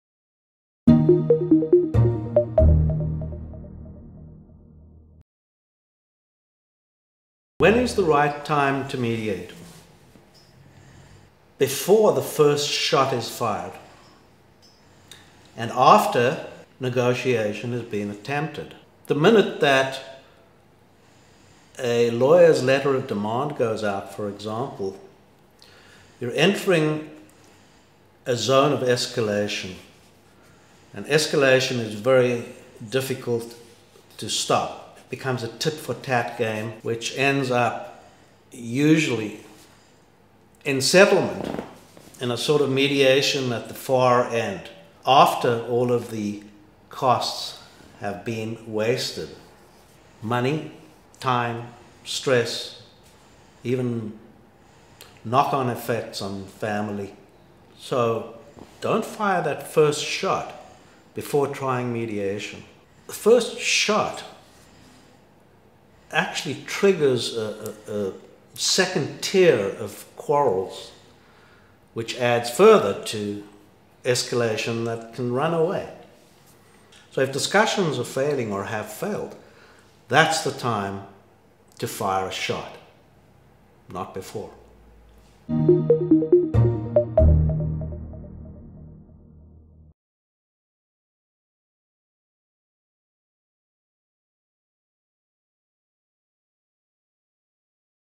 MEDIATION | WHEN is the best time for it?
After negotiation has failed but before any ultimatums are given.
Over 90% of litigated cases settle before getting to court; most of them after disproportionate amounts of money have been spent. Additionally, time gets wasted, and stress levels often spill over to others – including family.

To say that South Africa’s judicial system is inefficient is a colossal understatement.

A four-year wait to get to court is not unheard of, and judgements are frequently inconsistent, which makes it difficult to weigh up the odds of winning.

Rivalry often becomes a case of “who loses less”! Going straight from failed dialogue to anything that is adversarial is the waste of an opportunity to get a matter sorted out – even if not optimally – and move on. It is almost always the least costly way to go.

Firing the first shot sets off a reaction, and counter-reaction, and counter-reaction … until you’re in a conflict spiral that is consuming, and much more difficult to control and budget for.

Mediation at that point is a smart move.

It is a no-risk, non-binding, confidential, without prejudice, informal, and cheap alternative, with a 75% plus success rate. Moreover, nothing you have in your back pocket, whether advantages, weaknesses, or strategy, is diminished. Nothing that you don’t want to divulge to the other side gets disclosed to the other party. It is the mediator that you confide in only. Therefore, if you decide not to settle in Mediation, all of your trump cards remain intact and can go right along with you to Arbitration or Litigation – nothing gets ‘lost’.

It’s not easy to convince your opponent to enter Mediation.

Misconceptions abound as to what Mediation actually is.
It is often confused with Arbitration, which is an adjudicative process much like a court trial. That is quite the opposite of Mediation.

Getting complete information to your opposition is your primary challenge.

Because your lawyer acts in your best interest, she/ he is bound to advise of the opportunities that early settlement provides, and the various ways that that can be achieved. Litigation is valuable, but not before a Mediative process has been exhausted. So it is important that your lawyer is on board and knowledgeable about Mediation.

It is preferable to have your lawyer with you in Mediation.

She/ he will act as a sounding board, and the perfect partner to reality test your assumptions of what not settling means. Your lawyer is best qualified to draw up settlement agreements on the spot and capable of simplifying documentation so that agreements don’t fall out of bed.

Courts are starting to demand Mediation between disputants before trial and refusal may result in penalties. See Court ordered Mediation is becoming a reality.

In summation, STOP before firing the first shot

Offer to enter Mediation with your opponent. It is not a show of weakness, but rather one of good business sense.

"One of the best ways to persuade others is with your ears-by listening to them" ~ Dean Rusk